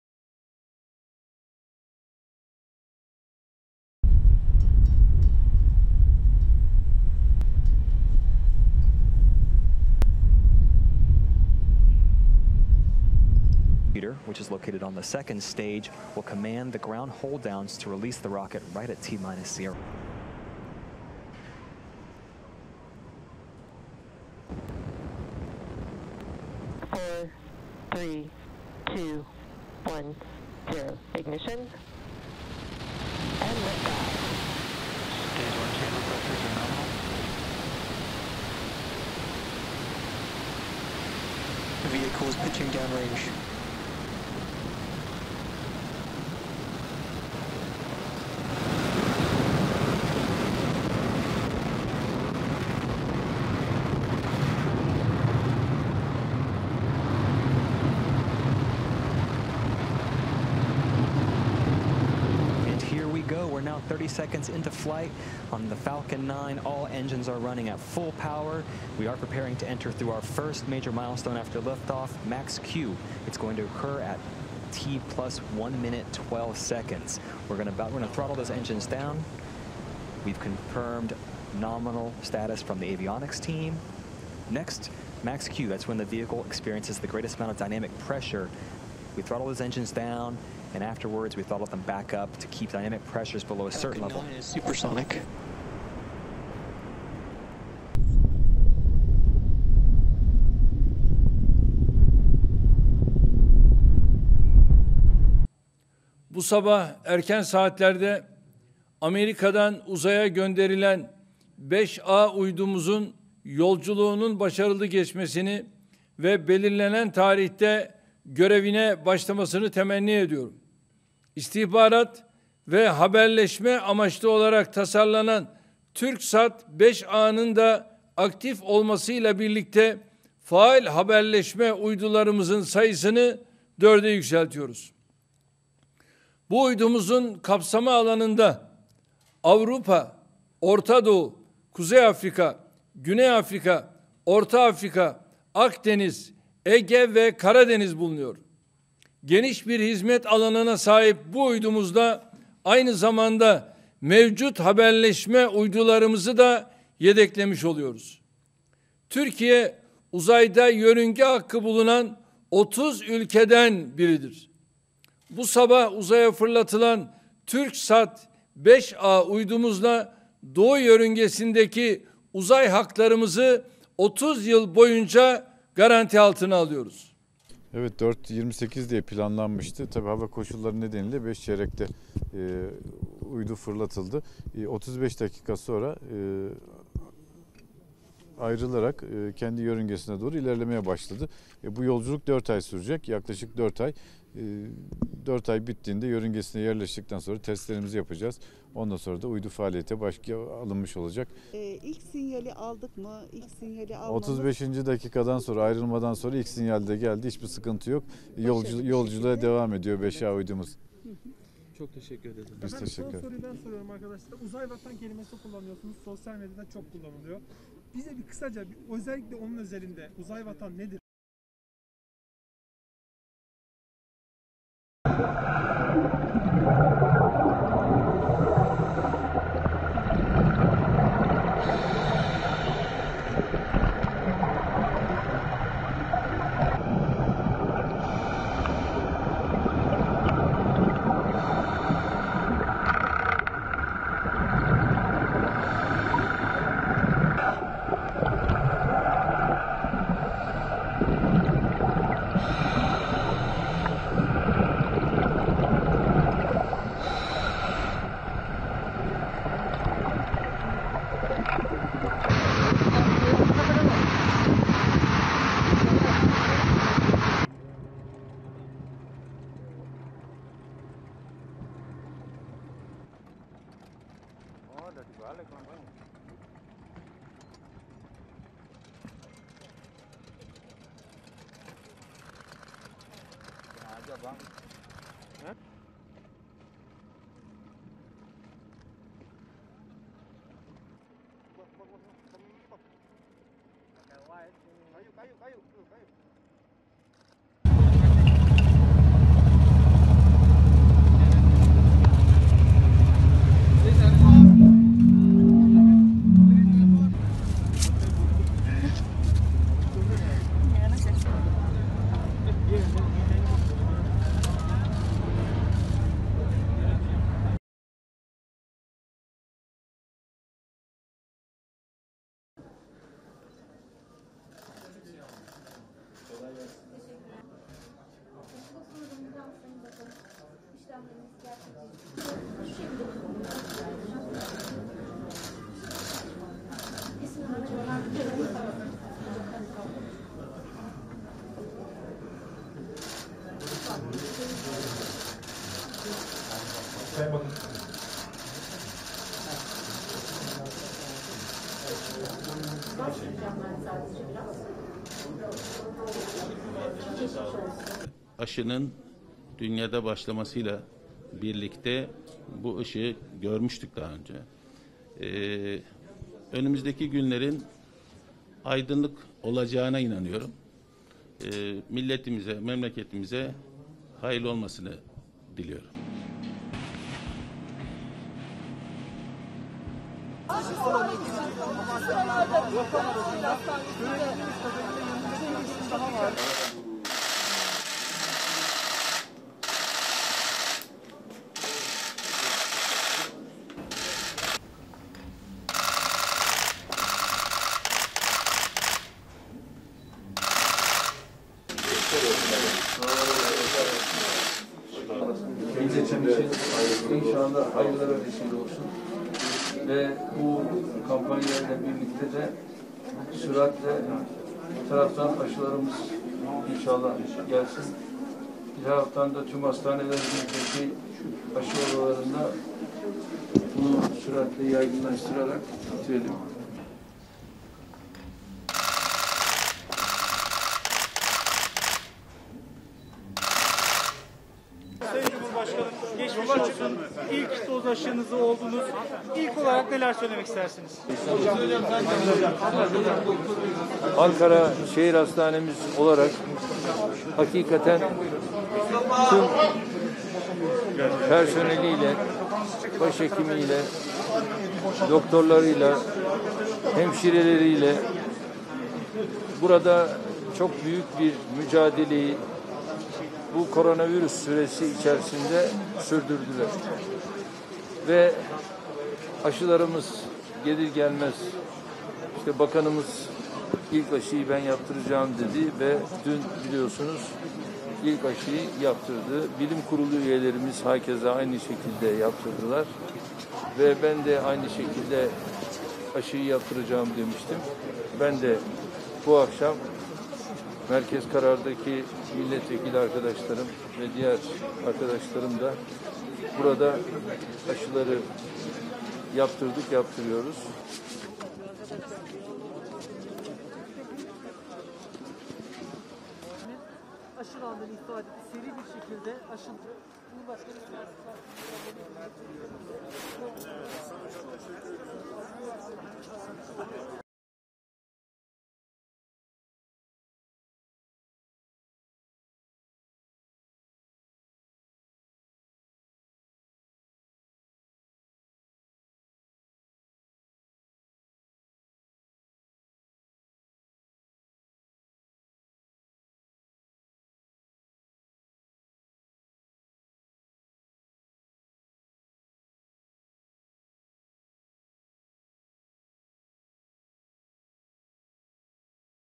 2021 Yılında Yaşanan olaylar OCAK (2)
00:00 Türksat 5A uydusu, ABD'nin Florida eyaletinde bulunan Cape Canaveral Üssü'nden uzaya fırlatıldı.

03:42 Endonezya'da kalkıştan yaklaşık 5 dakika sonra iletişimin kesildiği "SJ182" sefer sayılı iç hat seferi yapan yolcu uçağı, başkent Cakarta'nın açıklarındaki Laki ve Lancang Adası arasındaki noktaya düştü

05:18 Çinli Sinovac firmasınca üretilen CoronaVac aşısına Türkiye İlaç ve Tıbbi Cihaz Kurumu (TİTCK) tarafından "Acil Kullanım Onayı" verildi. Aşının ilk dozu Ankara Şehir Hastanesi'nde uygulanmaya başlandı. İlk olarak Sağlık Bakanı Fahrettin Koca ile Koronavirüs ve Toplum Bilim Kurulu üyelerine aşı yapıldı.

06:23 Cumhurbaşkanı Erdoğan, AK Parti Merkez Karar ve Yönetim Kurulu (MKYK) toplantısının ardından Ankara Şehir Hastanesi'ne gelerek Kovid-19 aşısının ilk dozunu yaptırdı.